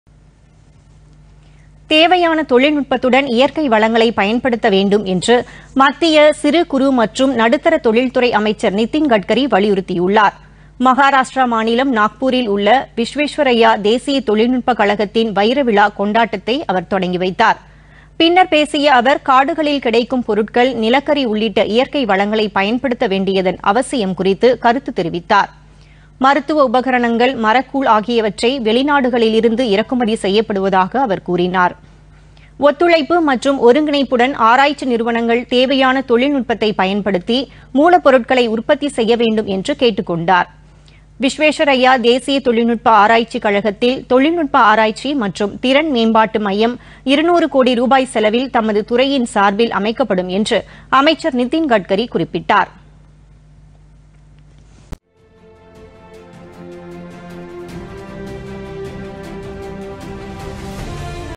இயற்கை வளங்களை பயன்படுத்த வேண்டும்
தேவையான தொழில்நுட்பத்துடன் இயற்கை வளங்களை பயன்படுத்த வேண்டும் - மத்திய சிறு குறு மற்றும் நடுத்தர தொழில்துறை அமைச்சர் நிதின் கட்கரி வலியுறுத்தல்